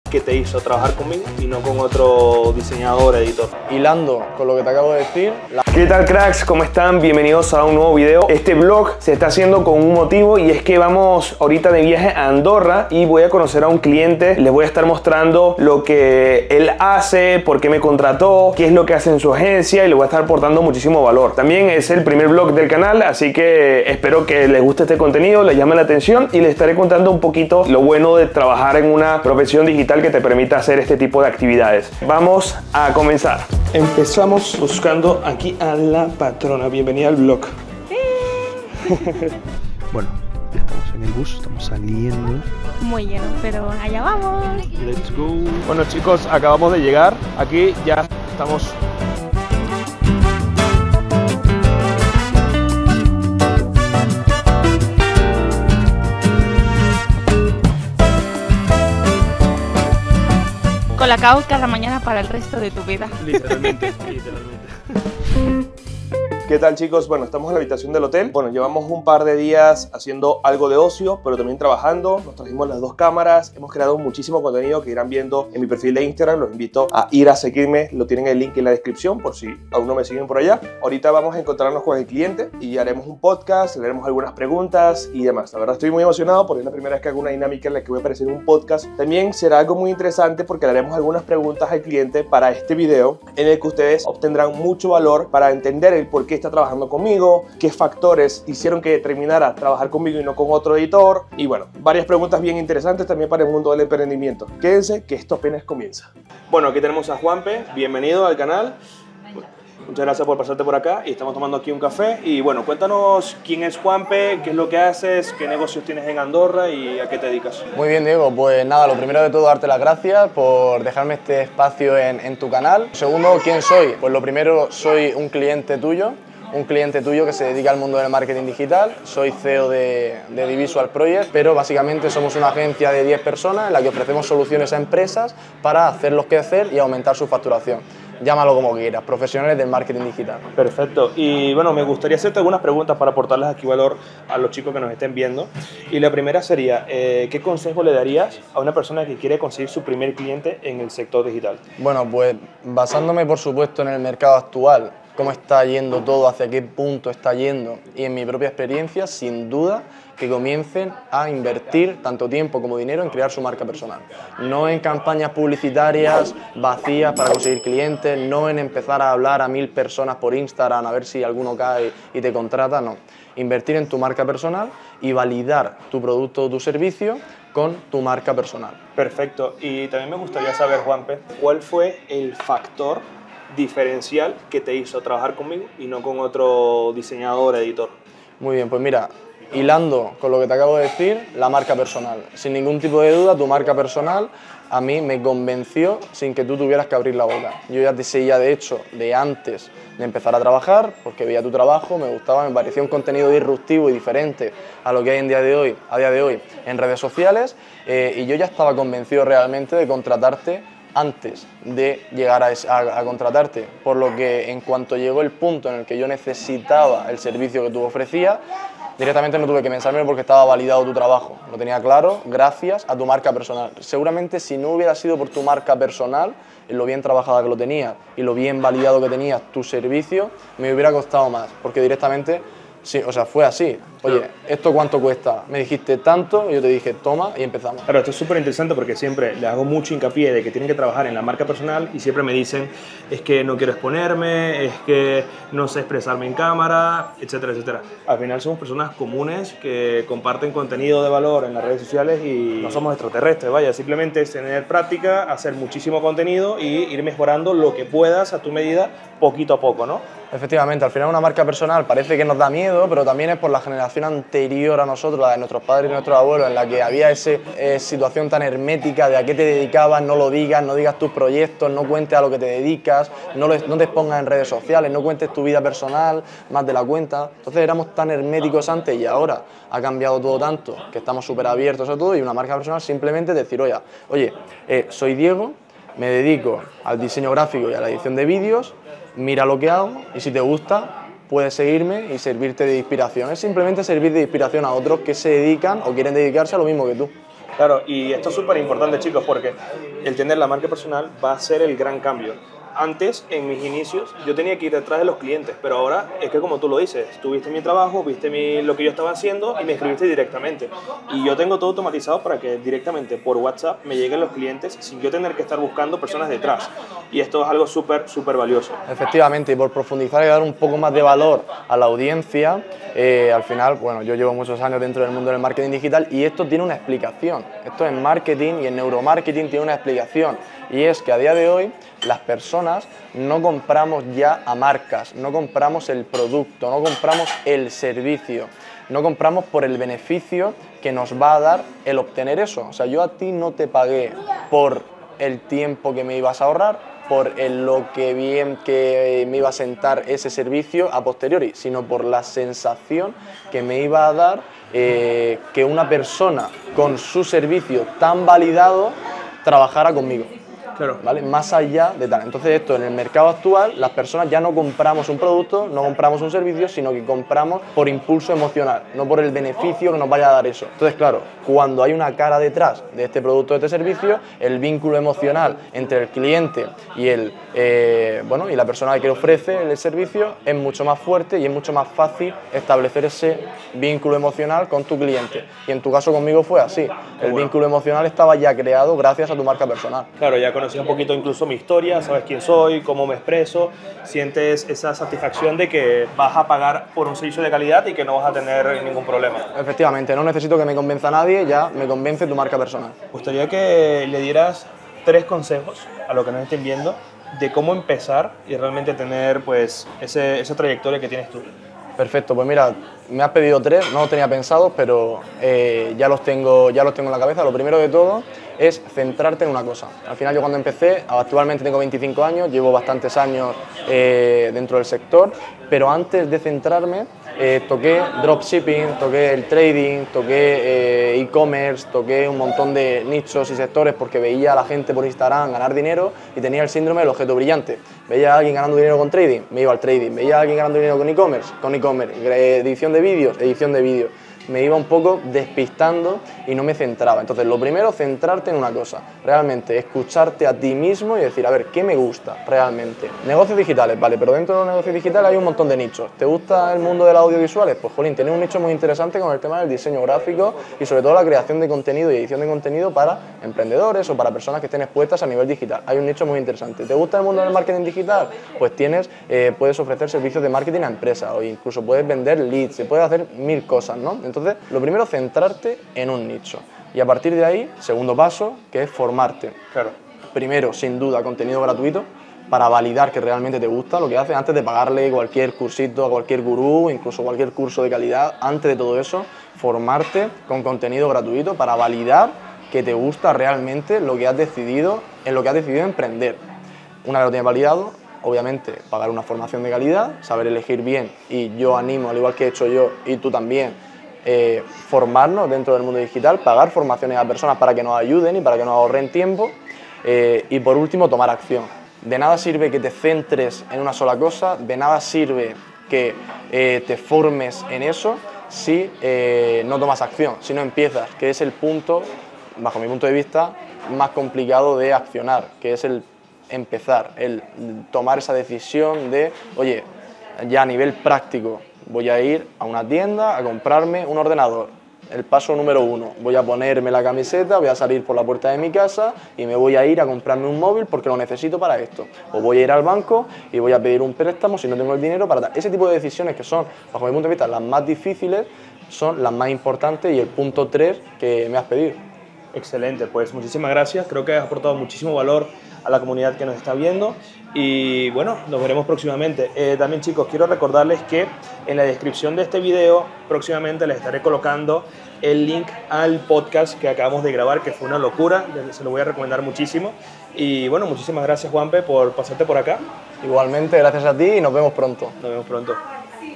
Viajo a Andorra: Conociendo a un Cliente | Primer VLOG⚡
En este video les comparto la experiencia de ir a conocer presencialmente a un cliente, le editamos reels y videos de Youtube.

⚡Este es el primer VLOG del canal, déjame saber en los comentarios que te pareció ⚡